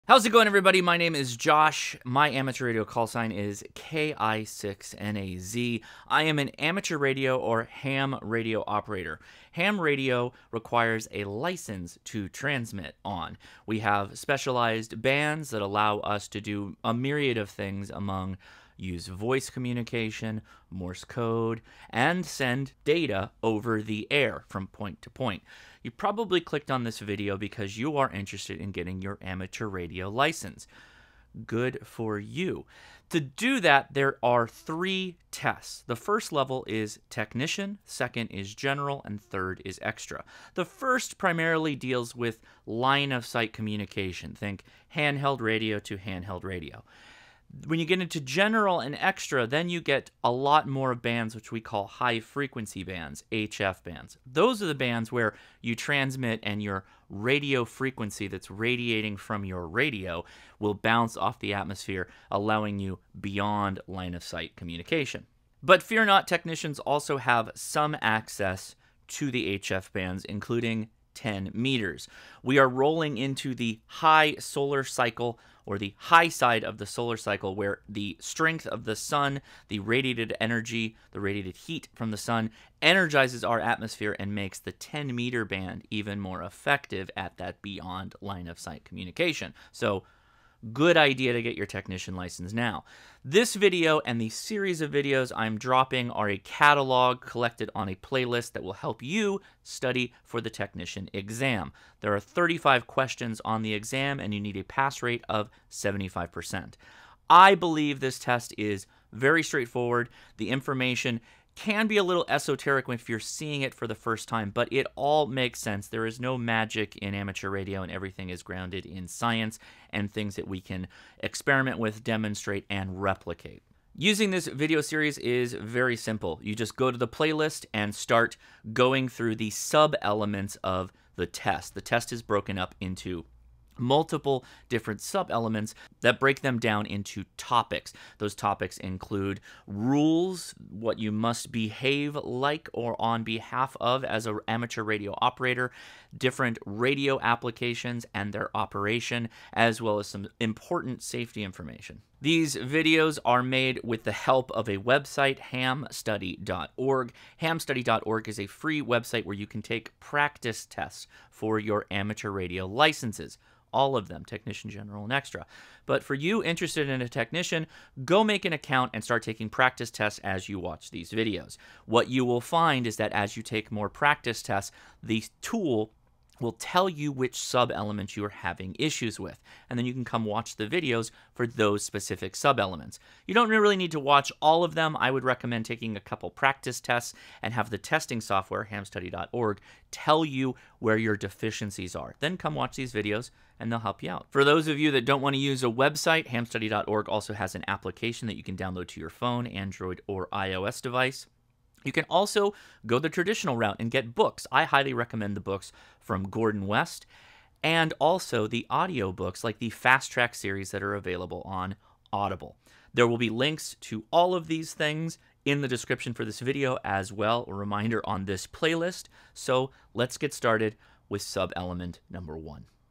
Ham Radio Technician Prep Intro (2022-2026)
I hope to help you get started on a rewarding journey into better understanding amateur radio.  Below are the links mentioned in the video.

Gordon West Study Book
The Fast Track to Your Technician Class Ham Radio License

📻You can find some of my favorite radios at GigaParts: The DigiRig can be found
📻Check out Radioddity ham radio radios and get a discount: Xiegu G90